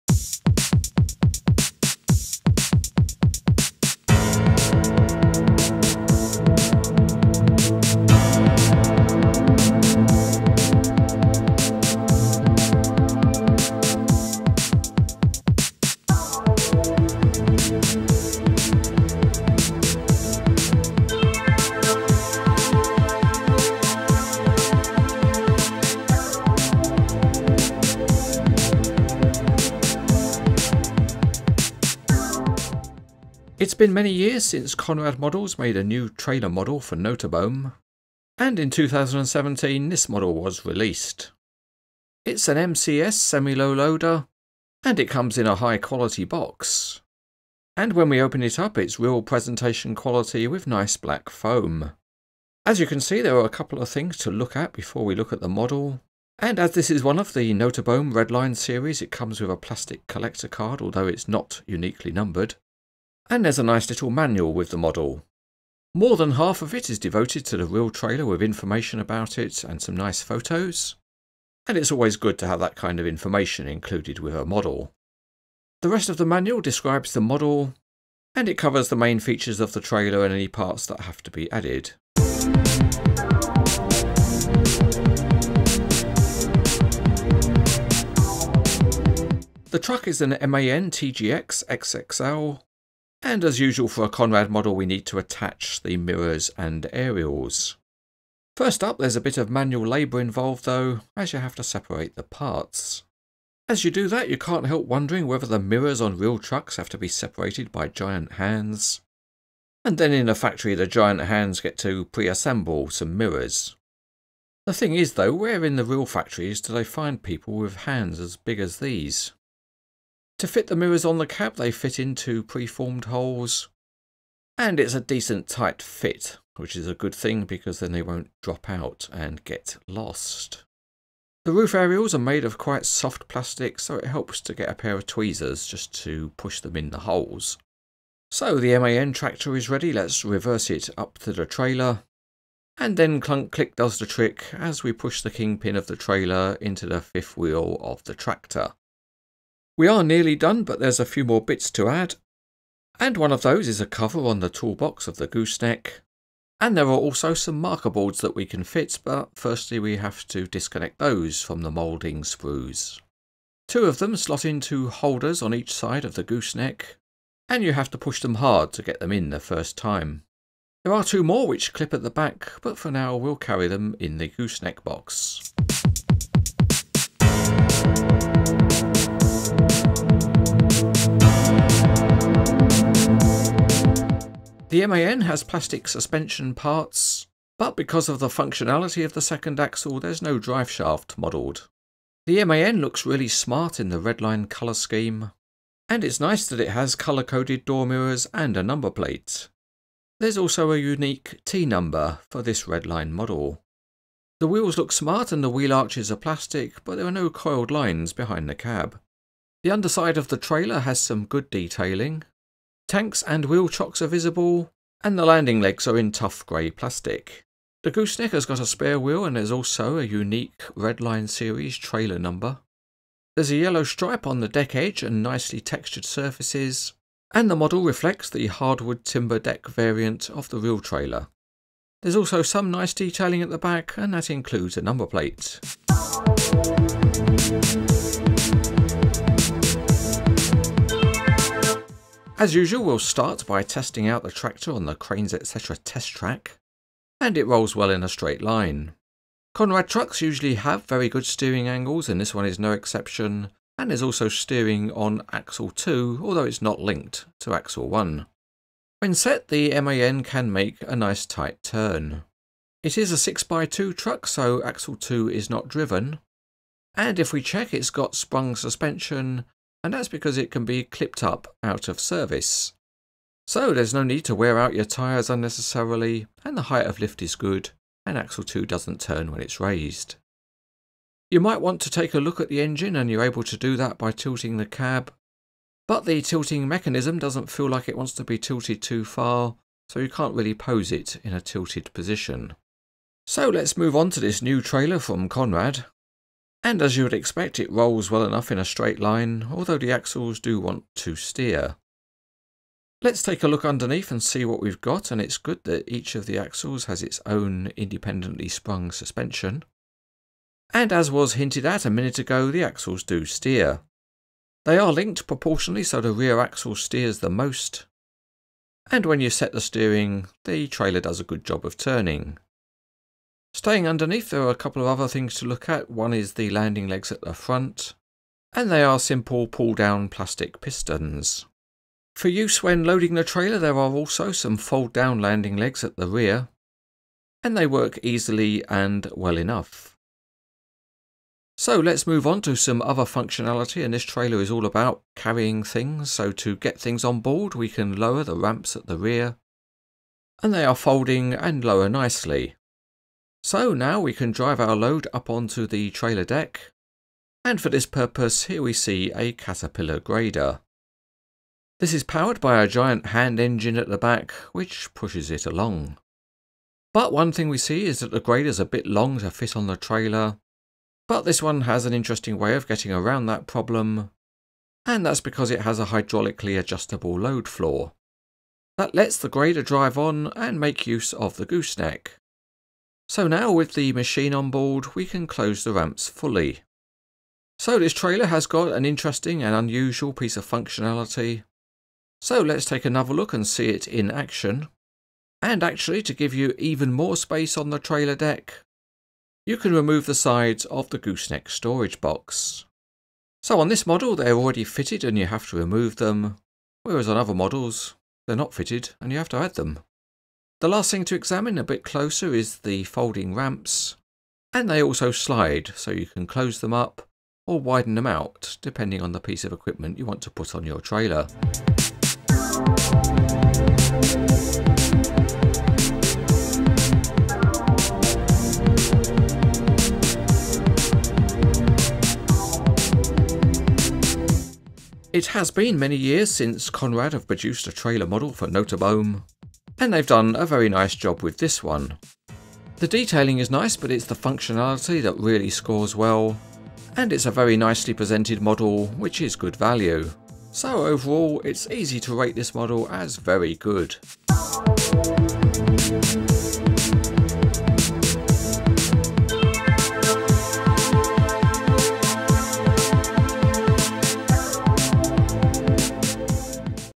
Conrad MAN TGX + Nooteboom MCOS Semi Low Loader by Cranes Etc TV
This is the Cranes Etc TV review of Conrad Model's 1/50 scale model of an MAN TGX XXL with a Nooteboom MCOS Semi Low Loader.  The model number is 76230.